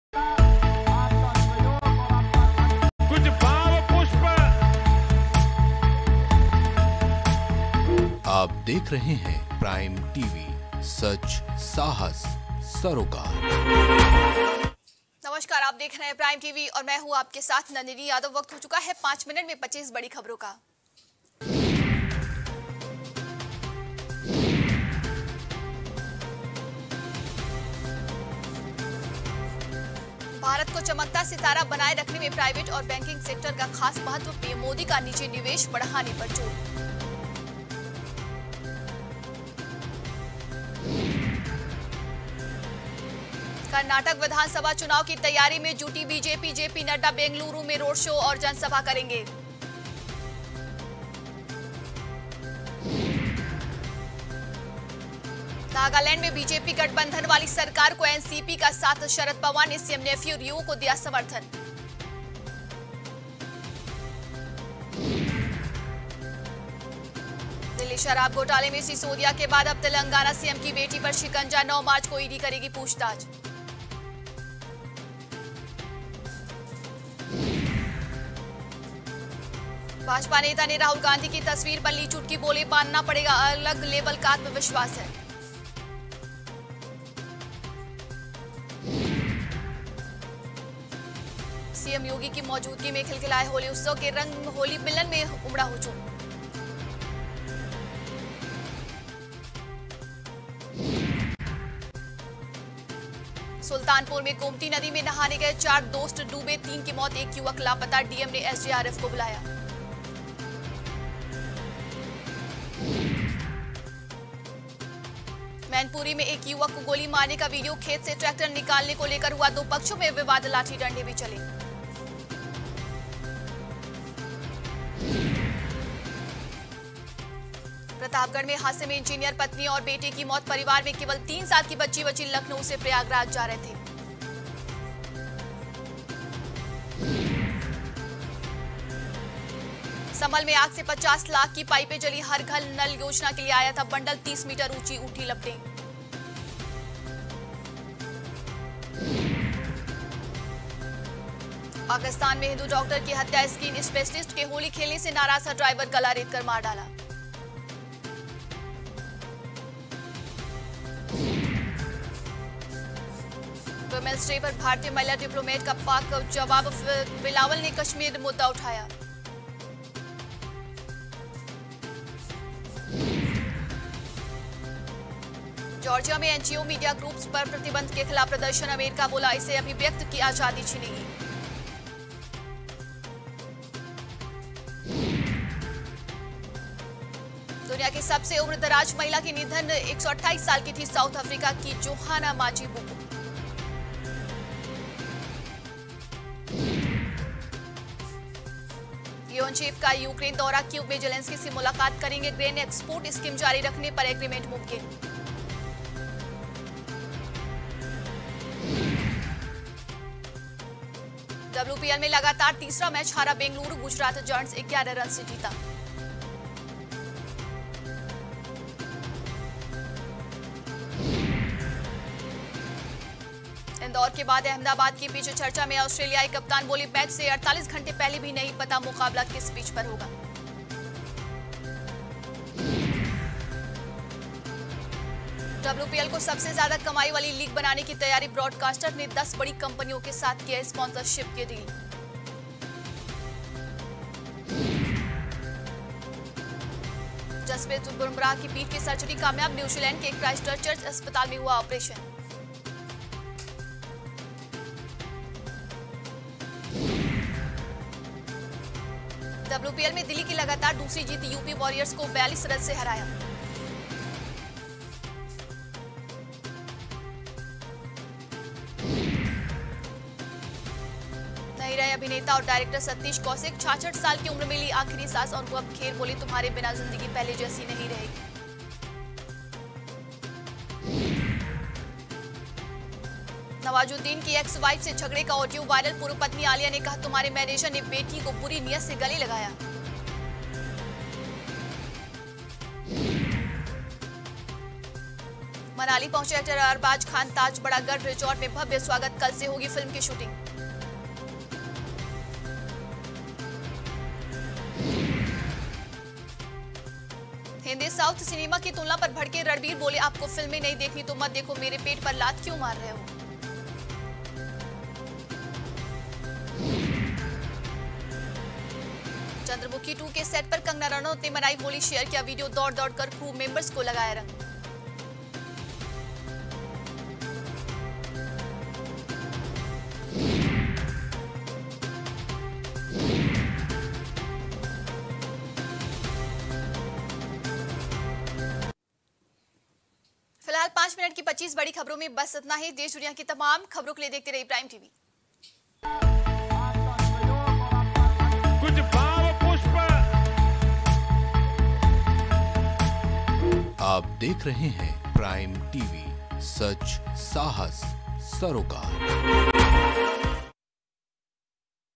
Liqour Scam : KCR की बेटी पर शिकंजा, Money Laundering को लेकर 11 मार्च को ED करेगी पूछताछ | Prime Tv
मनी लॉन्ड्रिंग को लेकर 11 मार्च को ED करेगी पूछताछ, शराब घोटाले में KCR की बेटी पर शिकंजा | Prime Tv